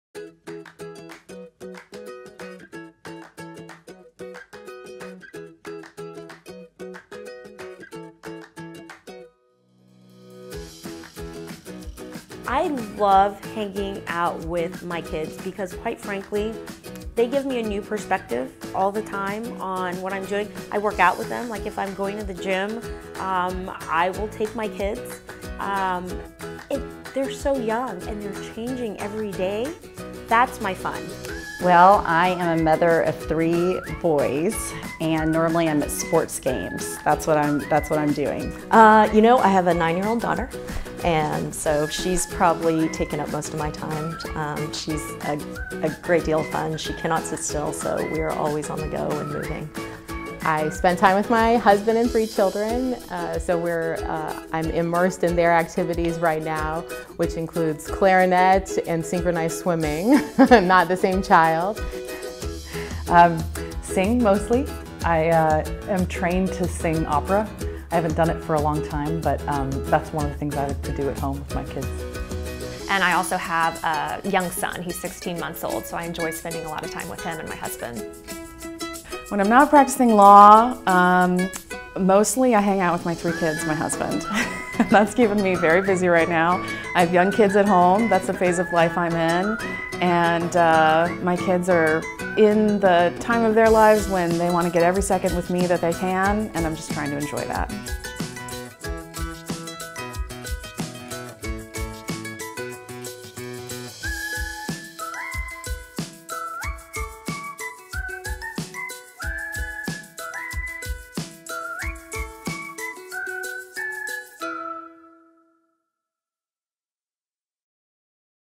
Shook Celebrates Our Working Mothers
Working Mother magazine has again ranked Shook, Hardy & Bacon as a top firm with the distinction of “Best Law Firm for Women” for 2020. This external review is based on the criteria of career flexibility, gender neutral family leave, retention and promotion of women, and much more. Watch as our Shook women partners explain the joys of motherhood and how they spend their time out of the courtroom.

The choice of a lawyer is an important decision and should not be based solely upon advertisements.